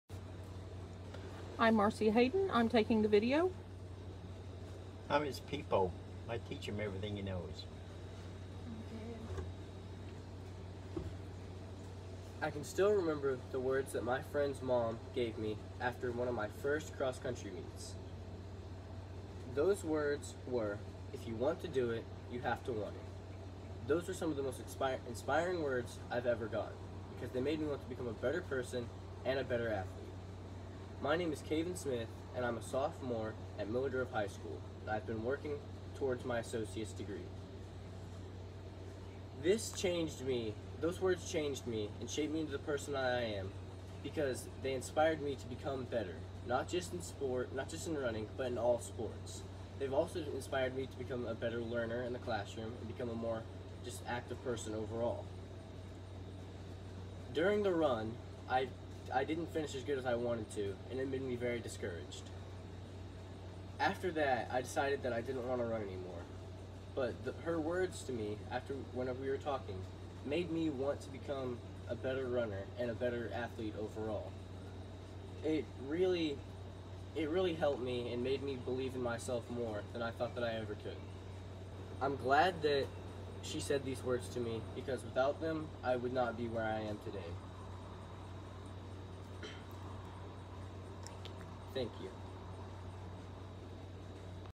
Event that shaped me into who I am
This is my speech for speech 1315 at PJC.
I know it’s kind of short but I was really nervous about this for some reason, I will definitely be using the Zoom method for the next one.